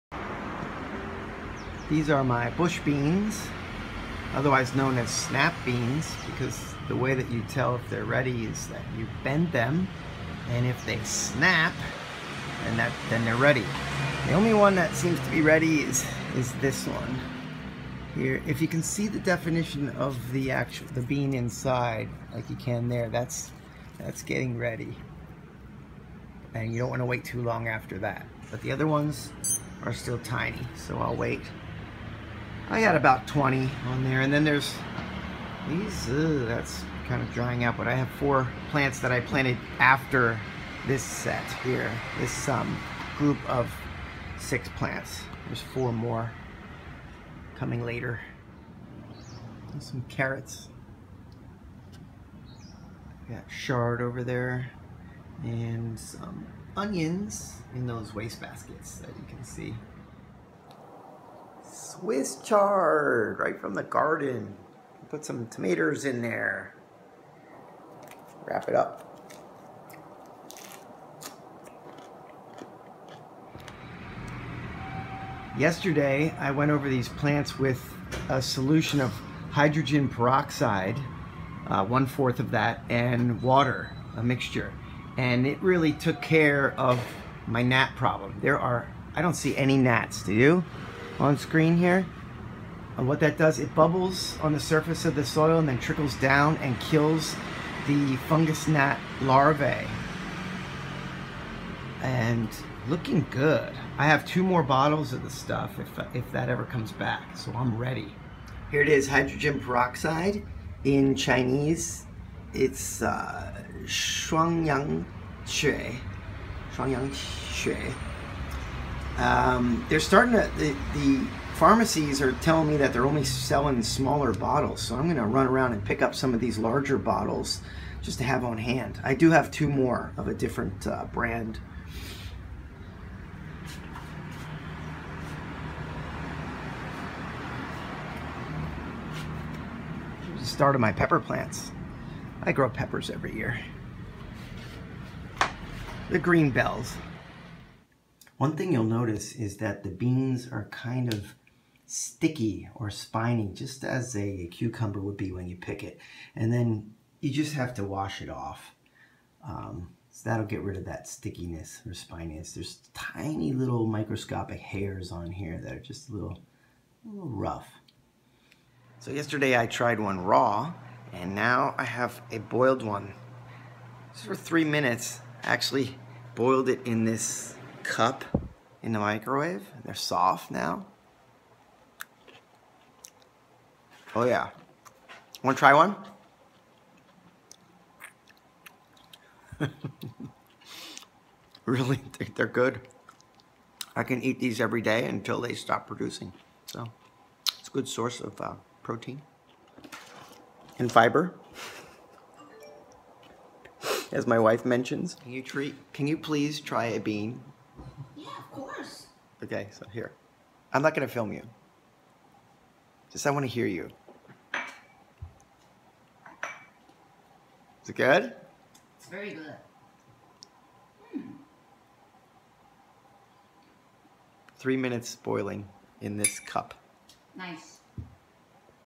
My First Bush Bean Harvest (& Fungus Gnat Solution)
Here is another FIRST for me!...BUSH BEANS (or snap beans). I planted about ten beans and then thinned it down to six plants in a tub. A few weeks later I planted four more in a pot. In this video I describe when to harvest them and a super-easy prep for a quick snack! Just boil them in a coffee mug in the microwave for three minutes and EAT!

Recently, I had been battling a fungus gnat problem.  However, it seems to be under control now after a solution of one-fourth hydrogen peroxide with water.  It kills the larvae.  I will place some sticky paper in each plant at the base of the stalks to monitor whether they come back.  If they do, I will just do another dousing.  In my research, it will not hurt the plants.   By the way, fungus gnat larvae can really do a number on your plant's root systems, so as soon as you see the gnats, it is a good idea to act fast!  Some of my swiss chard already fell victim!

I also show some of my other plants. I've got container carrots, swiss chard, and bell pepper plants, as well as my usually collection of the "ghost plant," or house leek.  (I did not talk about my pineapples because they will not be ready for another two years!)